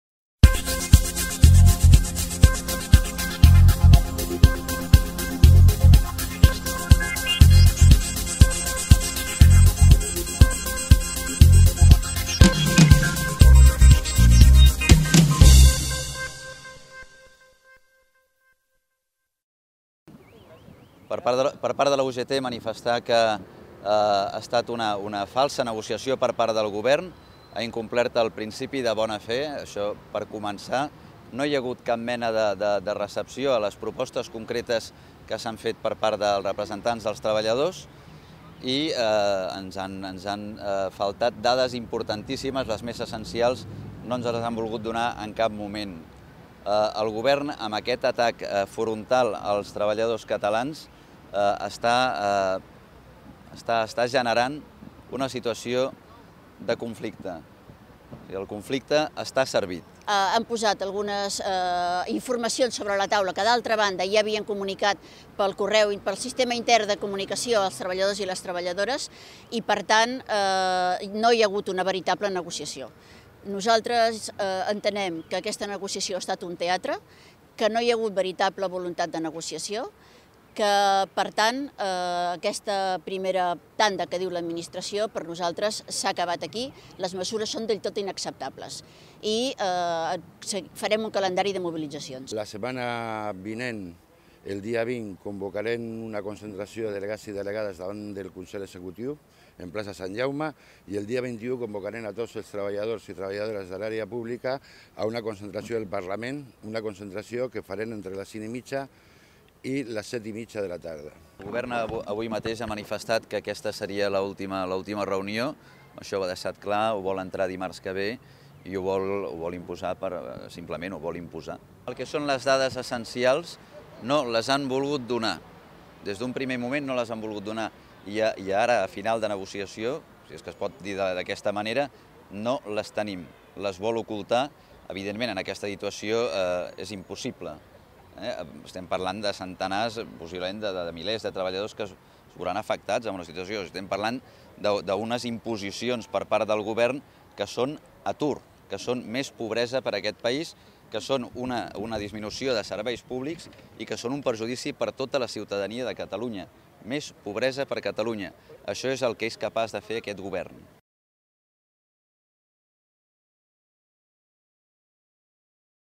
Sense acord a l'última reunió de la funció pública
La setena reunió entre govern i sindicats de la funció pública ha acabat sense acord. Per a la UGT de Catalunya, el contingut de la proposta és inacceptable. Per tant, totes les mesures d'aquesta proposta que s'apliquin són responsabilitat exclusiva de la Generalitat.